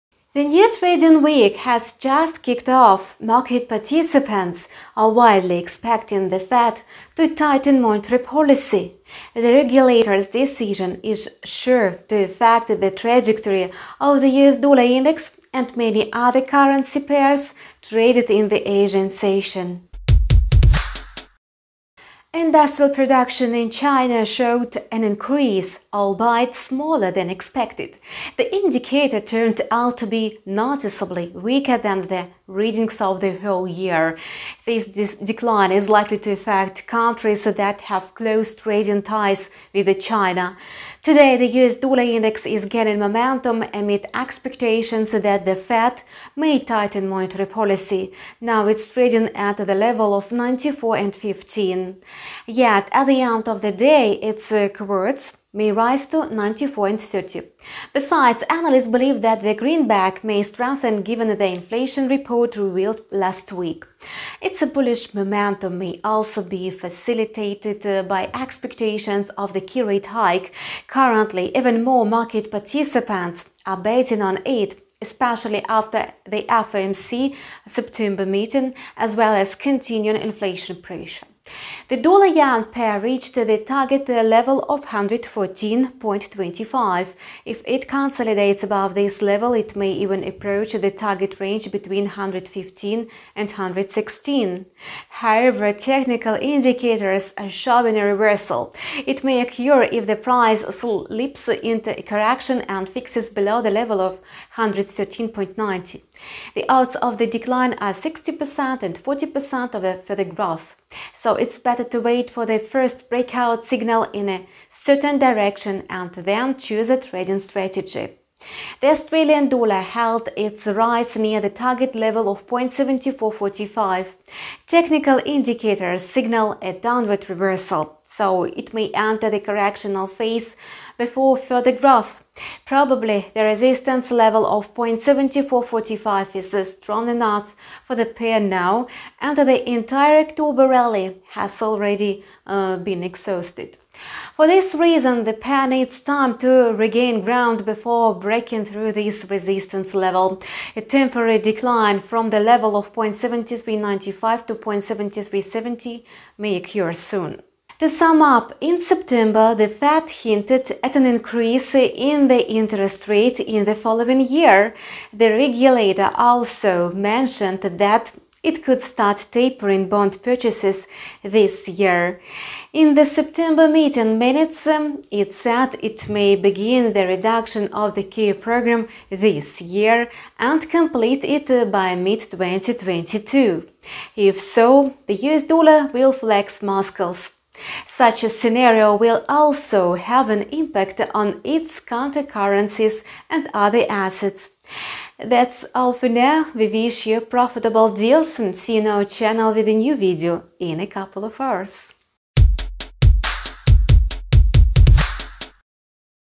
18.10.2021: USD may regain momentum; outlook for USD, USD/JPY, AUD/USD
The new trading week has just kicked off. Market participants are widely expecting the Fed to tighten monetary policy. The regulator’s decision is sure to affect the trajectory of the US dollar index and many other currency pairs traded in the Asian session.
Industrial production in China showed an increase albeit smaller than expected. The indicator  turned out to be noticeably weaker than the readings of the whole year. This decline is likely to affect countries that have close trading ties with China.
Today, the US dollar index is gaining momentum amid expectations that the Fed may tighten monetary policy. Now, it is trading at the level of 94.15. Yet, at the end of the day, its quotes may rise to 94.30. Besides, analysts believe that the greenback may strengthen given the inflation report  revealed last week. Its bullish momentum may also be facilitated by expectations of the key rate hike. Currently, even more market participants are betting on it, especially after the FOMC September meeting, as well as continuing inflation pressure.
The dollar/yen pair reached the target level of 114.25. If it consolidates above this level, it may even approach the target range between 115 and 116. However, technical indicators are showing a reversal. It may occur if the price slips into a correction and fixes below the level of 113.5. The odds of a decline are 60% and 40% of further growth. So, it is better to wait for the first breakout signal in a certain direction and then choose a trading strategy.
The Australian dollar held its rise near the target level of 0.7445. Technical indicators signal a downward reversal. So, it may enter the correctional phase before further growth. Probably, the resistance level of 0.7445 is strong enough for the pair now and the entire October rally has already been exhausted. For this reason, the pair needs time to regain ground before breaking through this resistance level. A temporary decline from the level of 0.7395 to 0.7370 may occur soon.
To sum up, in September, the Fed hinted at an increase in the interest rate in the following year. The regulator also mentioned that it could start tapering bond purchases this year. In the September meeting minutes, it said it may begin the reduction of the QE program this year and complete it by mid-2022. If so, the US dollar will flex muscles. Such a scenario will also have an impact on its counter currencies and other assets.
00:00 Intro
00:21 China’s Industrial Production
00:40 USDX
01:21 USD/JPY
01:58 AUD/USD